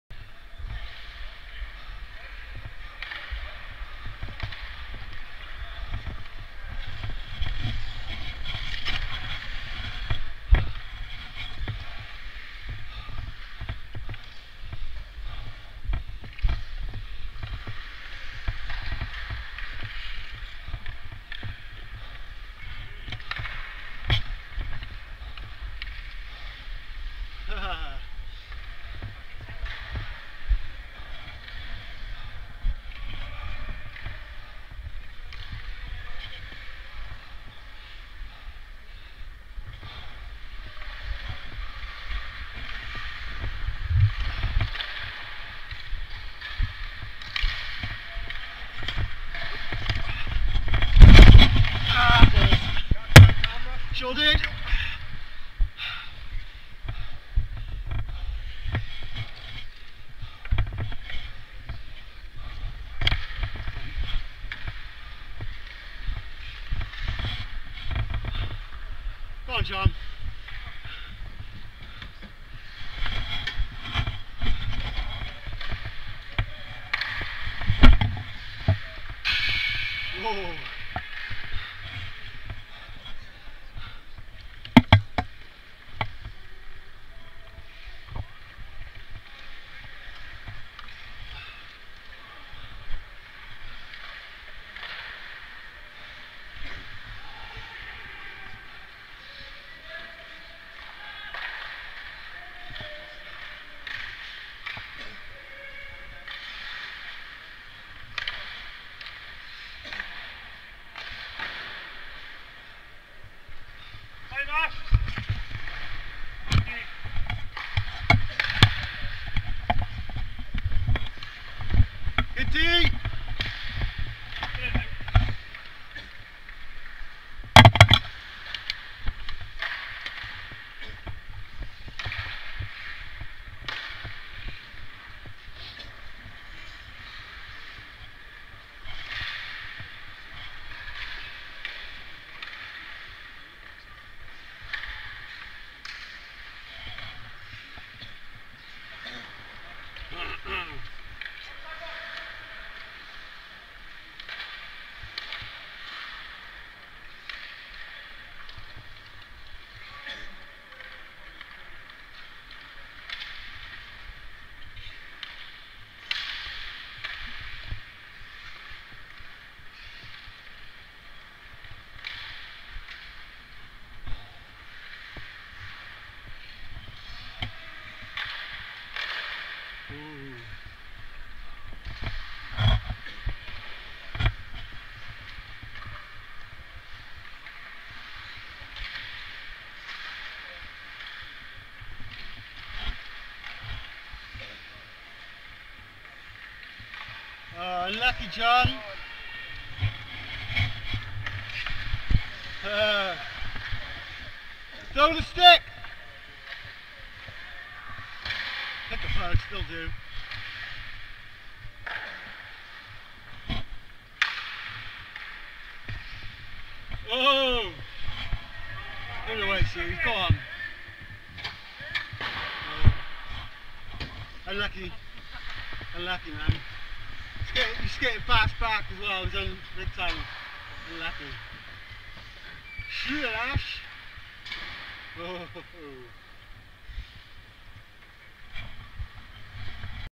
Test Video 18/12/2013
Random 4 min video from near the end of the session. Quality has been reduced so I could get this up tonight. Earlier footage is better before the ice gets torn up, more light is reflected so the colours are better. Enjoy!